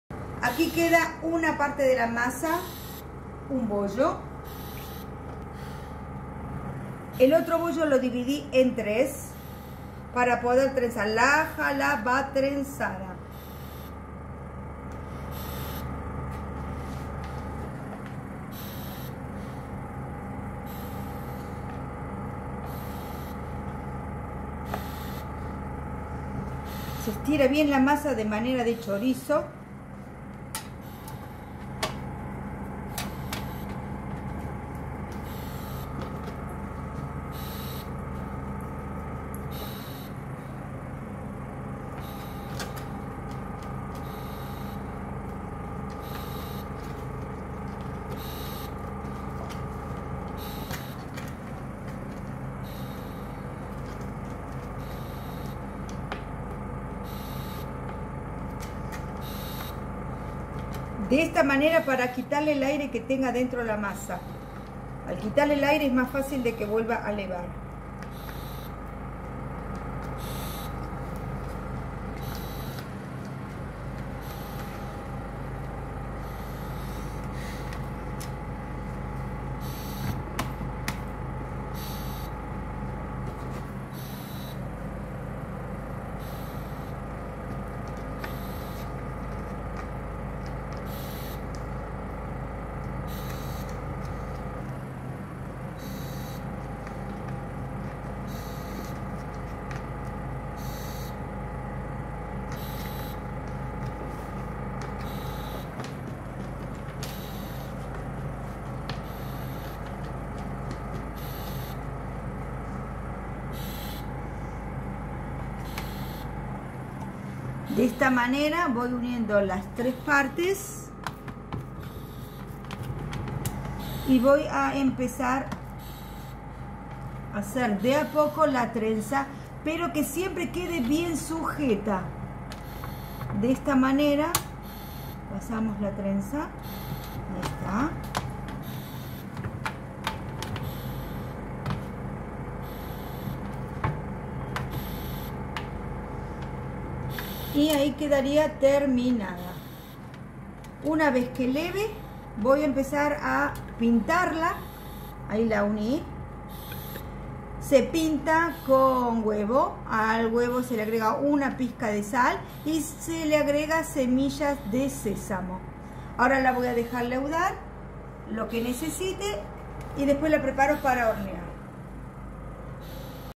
Armado de la Jala - pan trenzado parte 2
Segunda Jala  pan trenzado y dos Jalot pequeñas.